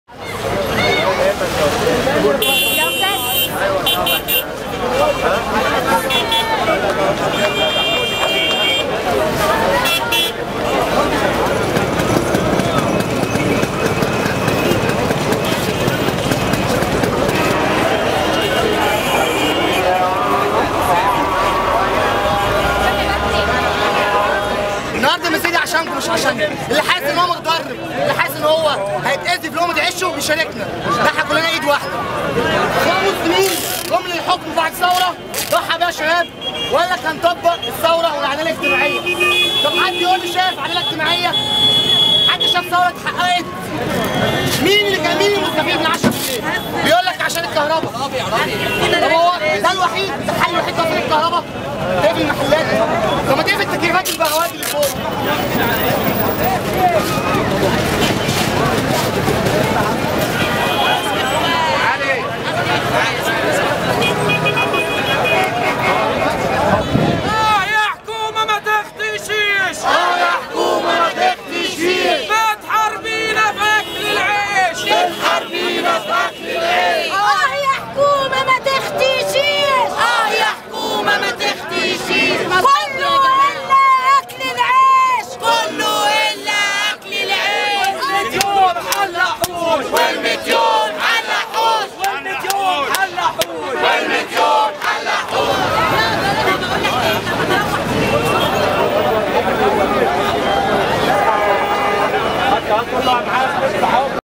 مظاهرت اكل العيش بميدان الشعرية
قام العديد من اصحاب المحلات بمظاهرات في ميدان الشعرية اعتراضآ علي قرار اغلاق المحال التجارية في الساعة العاشرة مسائآ بهدف خفض استهلاك الكهرباء واعلنوا رفضهم التام لهذا القرار الذي اعتبروه
"قطع عيش"